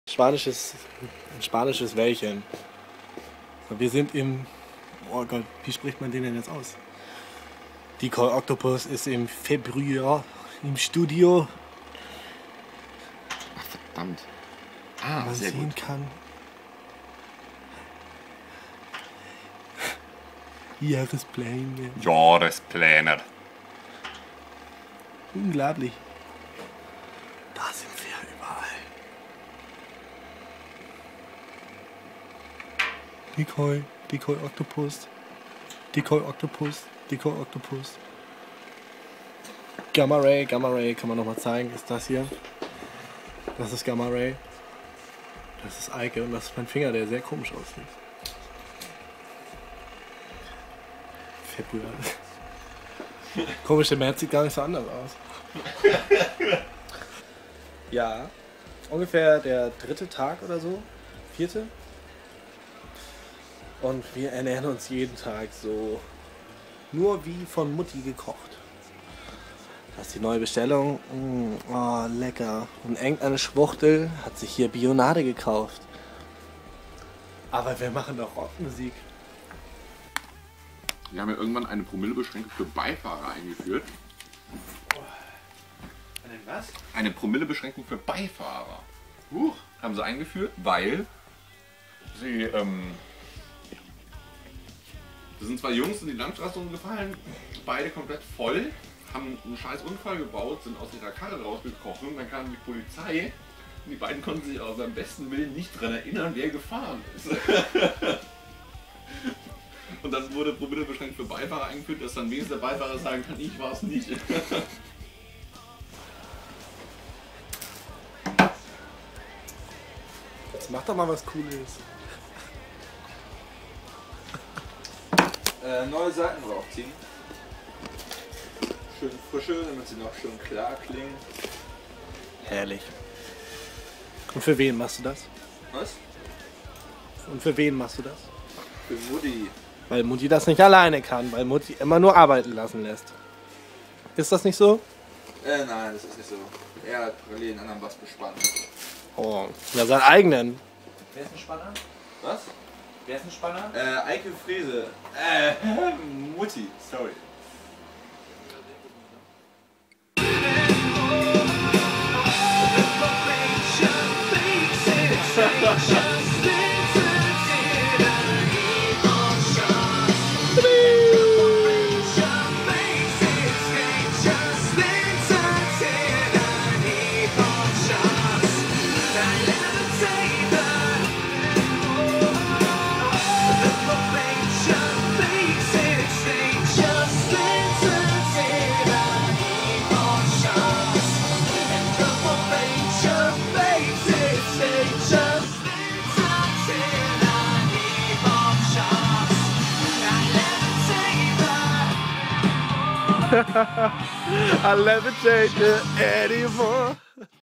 Decoy Octopus: Behind The Scenes #02 - Blank & Blanke
Unser Gitarrist und unser Bassist im Studio!